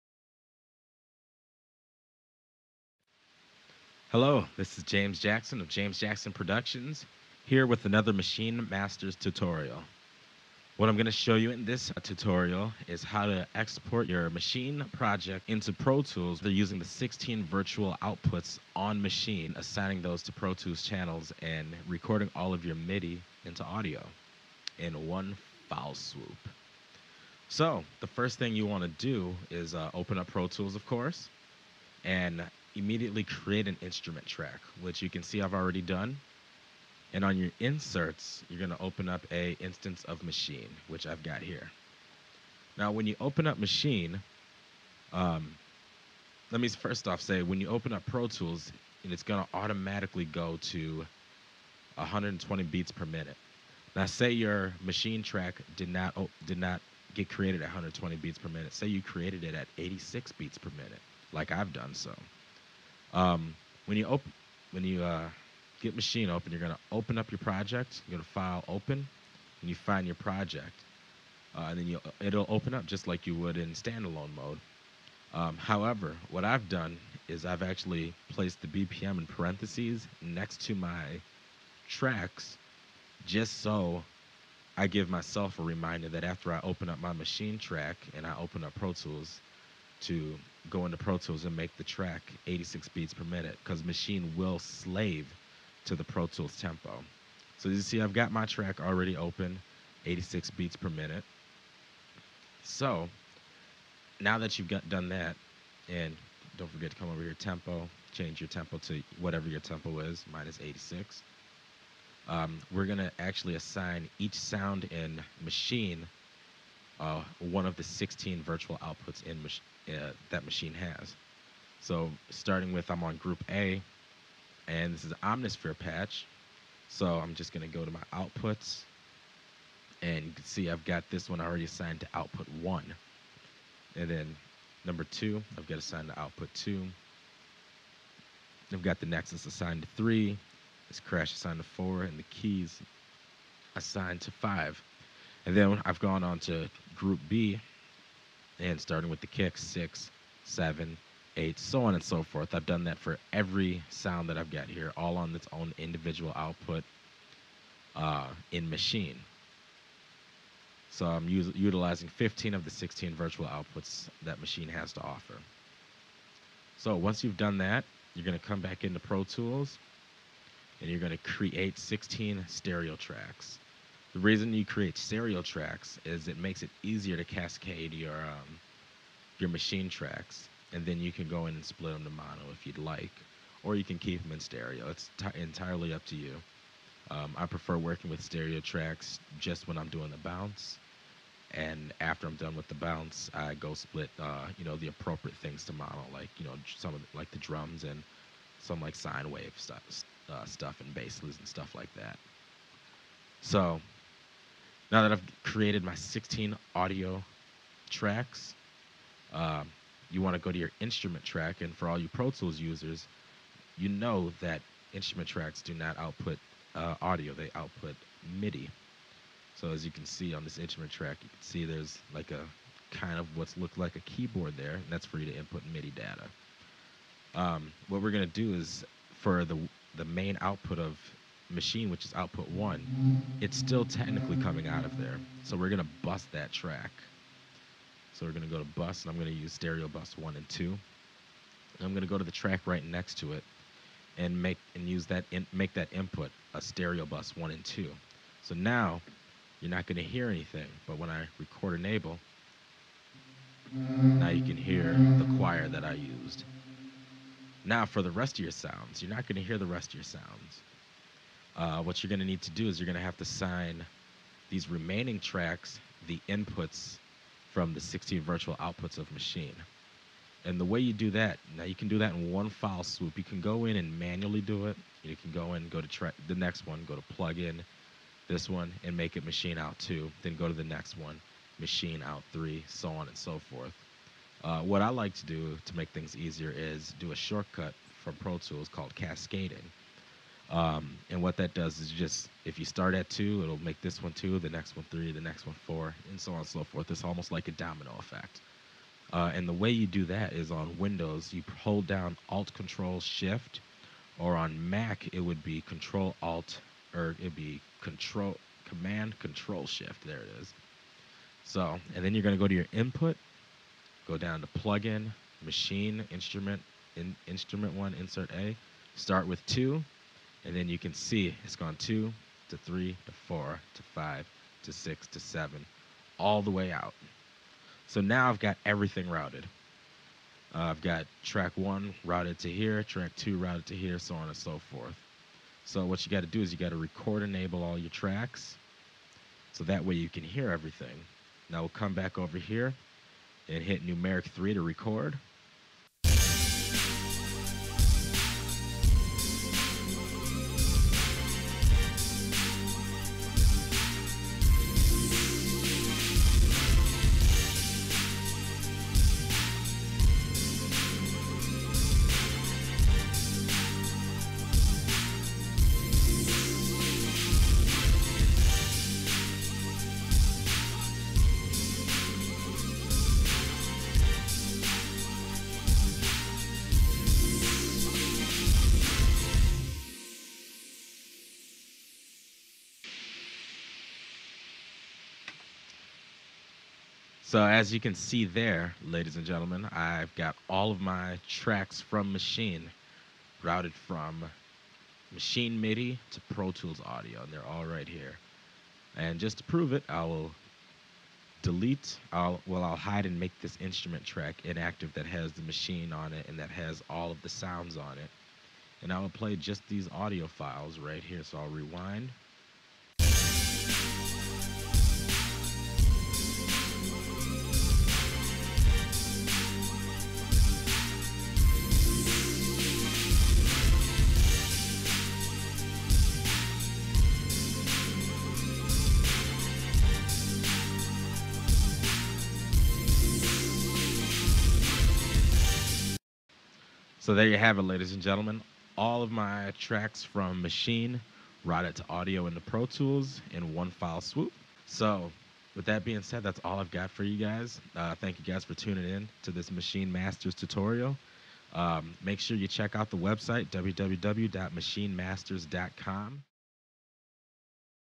Tutorial: Maschine Routing Audio into Pro Tools for Mixing
In this tip/tutorial J Jaxin shows you how to track out from maschine by routing audio to Pro Tools or your DAW of choice for further mixing and mastering.. Please join us